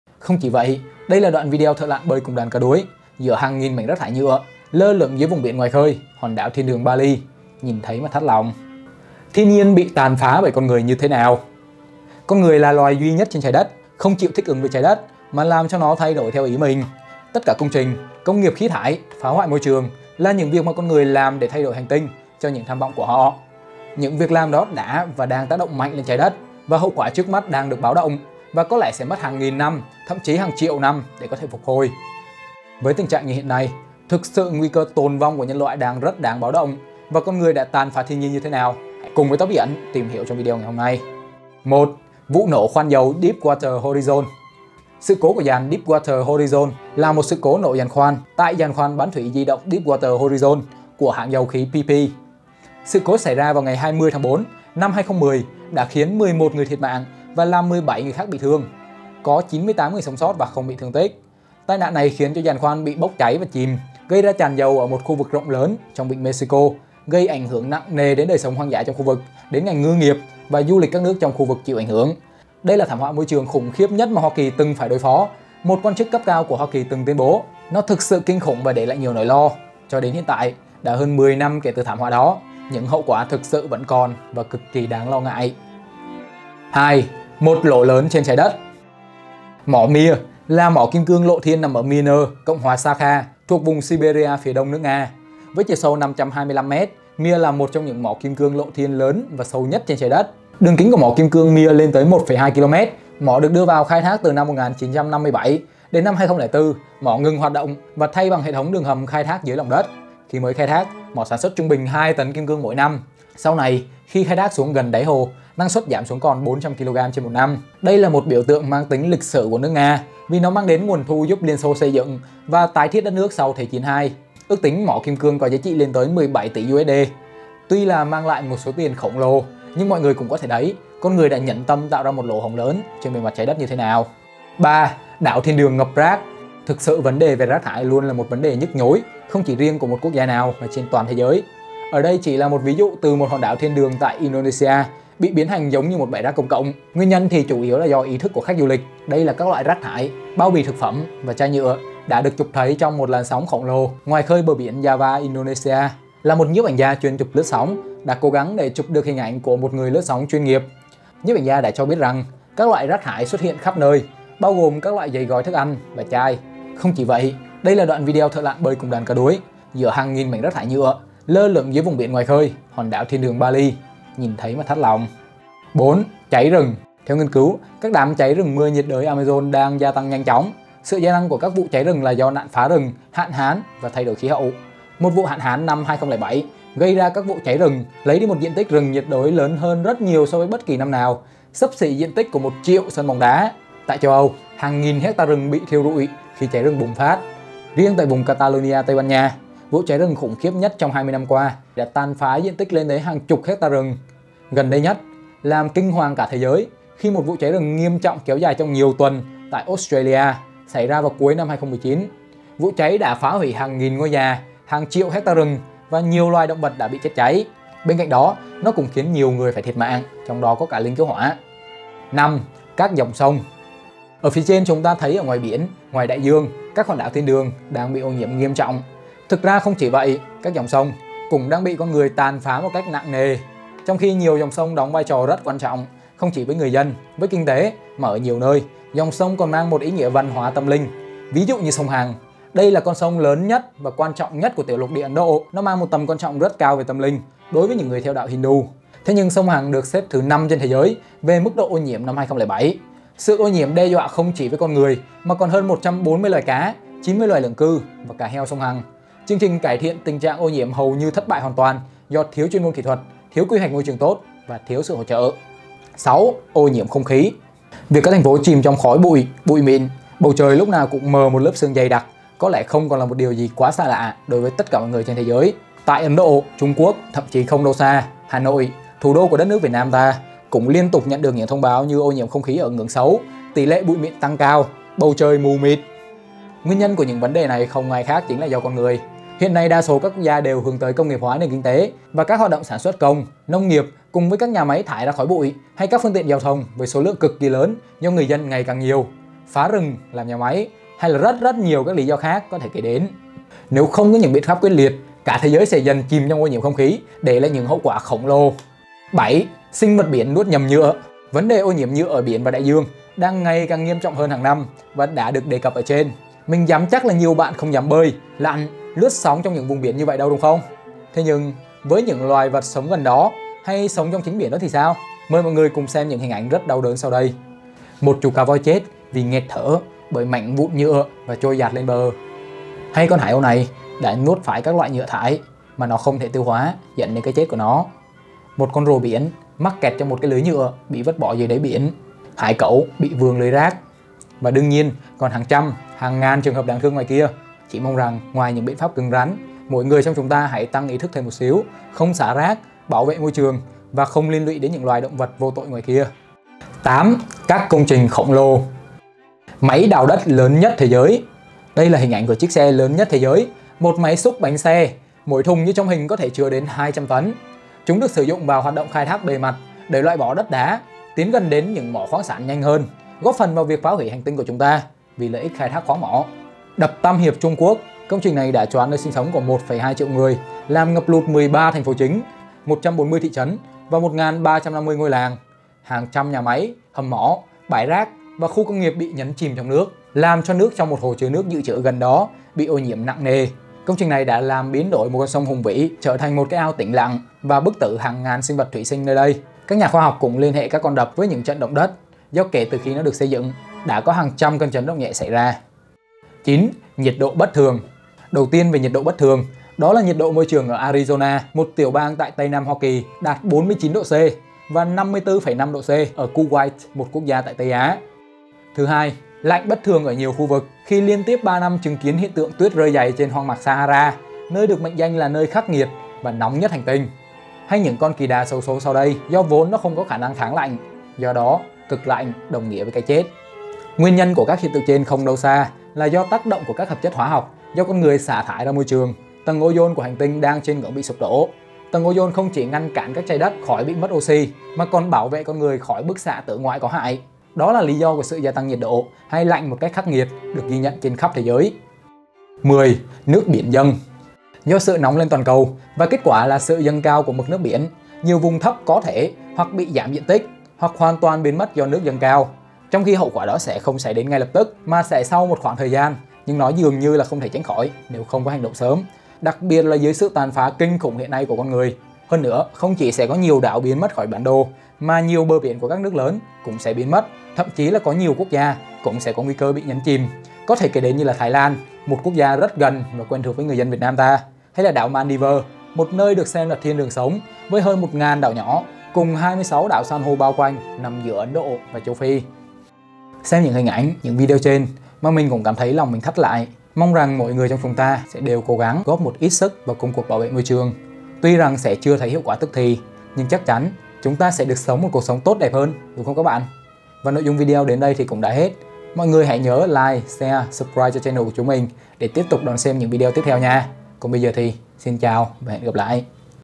Rơi Nước Mắt Trước Hình Ảnh Thiên Nhiên Bị Tàn Phá Bởi Con Người
THIÊN NHIÊN BỊ TÀN PHÁ BỞI CON NGƯỜI NHƯ THẾ NÀO
Con người là loài duy nhất trên trái đất không chịu thích ứng với trái đất mà làm cho nó thay đổi theo ý mình. Tất cả những công trình, công nghiệp khí thải, phá hoại môi trường... là những việc con người làm thay đổi hành tinh cho những tham vọng của họ. Những việc làm đó đã và đang tác động mạnh lên trái đất, và hậu quả trước mắt đang được báo động và có lẽ sẽ mất hàng ngàn thậm chí hàng triệu năm để có thể phục hồi. Với tình trạng hiện nay, thực sự nguy cơ tồn vong của nhân loại đang rất đáng báo  động, và con người đã tàn phá thiên nhiên thế nào, hãy cùng TBA tìm hiểu trong video hôm nay.
0:00 0. GIỚI THIỆU
0:41 1. VỤ NỔ DEEPWATER HORIZON
1:26 2. MỘT LỖ LỚN TRÊN TRÁI ĐẤT
2:14 3. ĐẢO THIÊN ĐƯỜNG NGẬP RÁC
2:55 4. CHÁY RỪNG
3:41 5. CÁC DÒNG SÔNG
4:25 6. Ô NHIỄM KHÔNG KHÍ
5:09 7. SINH VẬT BIỂN NUỐT NHẦM NHỰA
5:56 8. CÁC CÔNG TRÌNH KHỔNG LỒ
6:46 9. NHIỆT ĐỘ BẤT THƯỜNG
7:32 10. NƯỚC BIỂN DÂNG